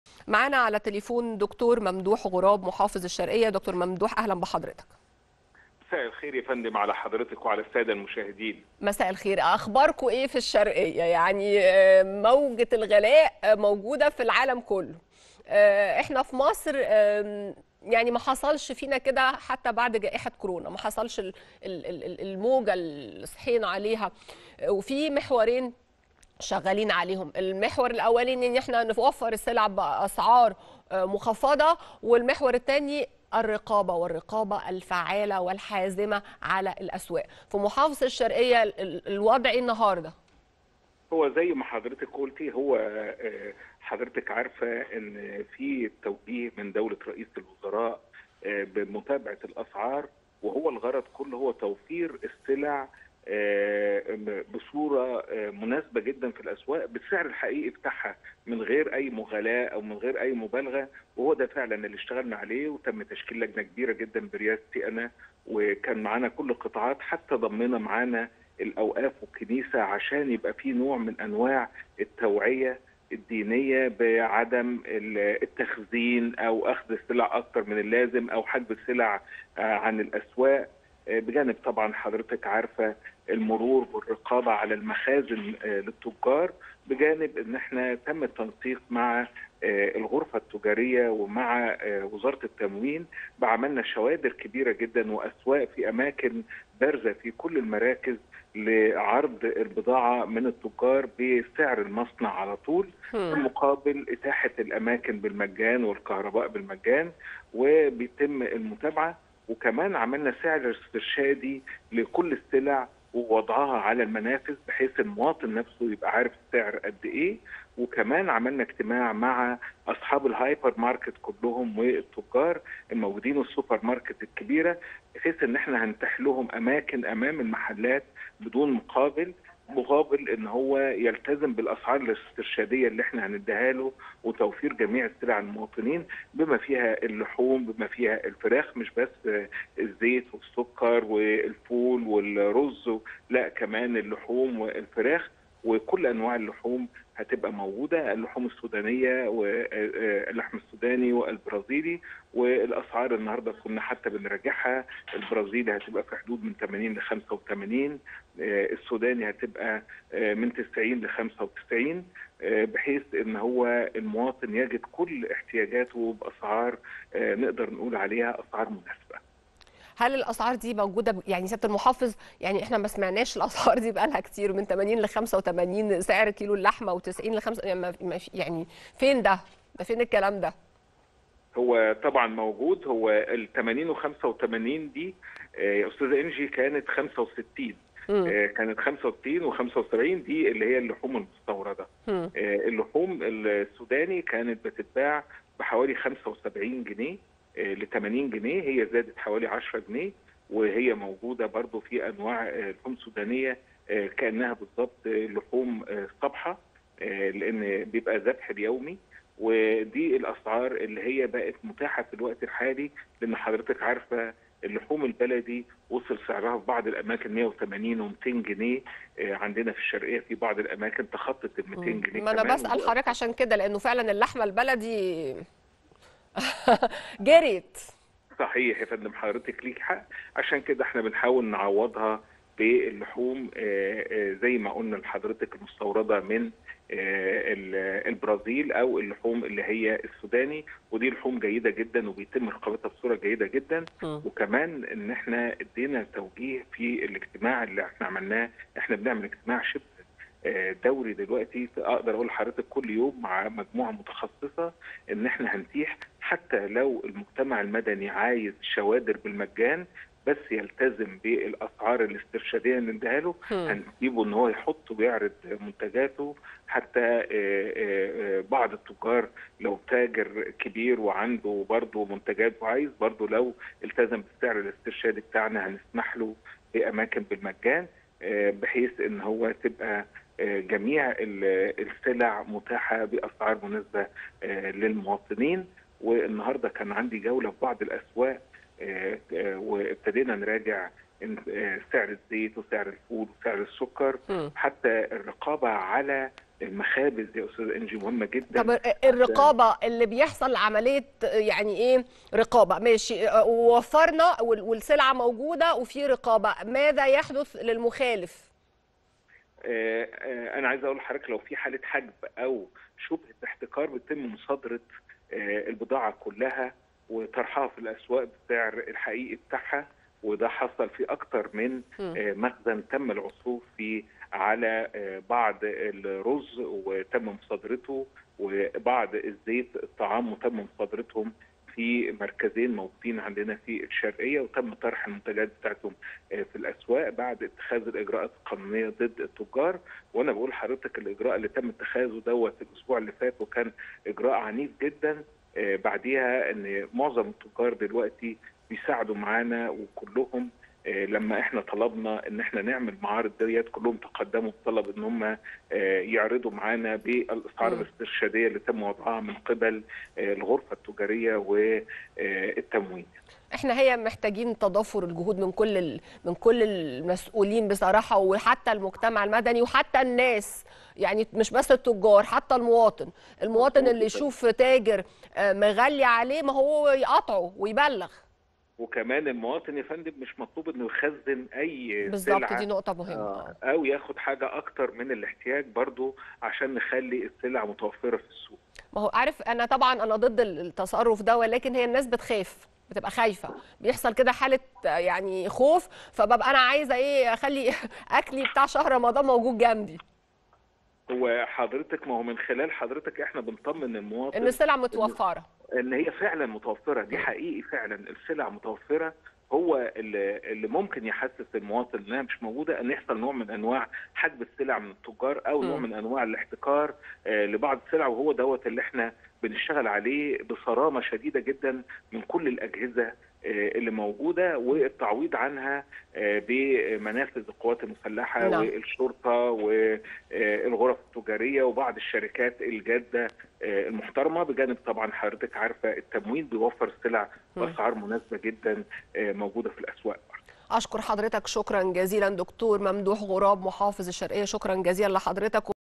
"محافظ الشرقية"، يوضح الاجراءات التي تم إتخاذها مع أصحاب الهايبرات، و أسعار اللحمة المستوردة مُخفض
د. ممدوح غراب "محافظ الشرقية"، يوضح الاجراءات التي تم إتخاذها مع أصحاب الهايبرات، لمواجهة الغلاء.. و أسعار اللحمة المستوردة مُخفض !